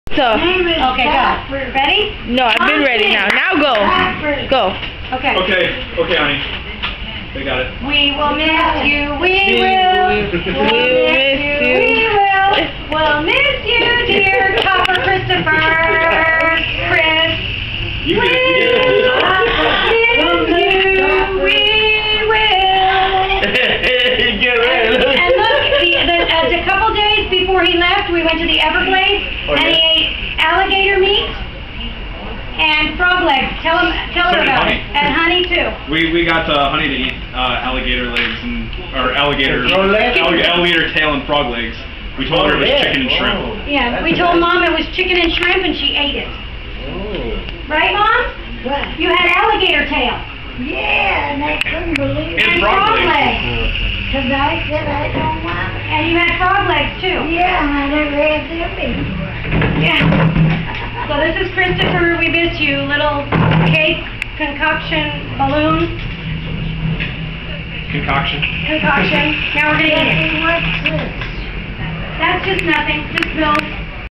More of Christopher's Going Away Party
We will miss you, we will. More of Christopher's going away party with Mom!!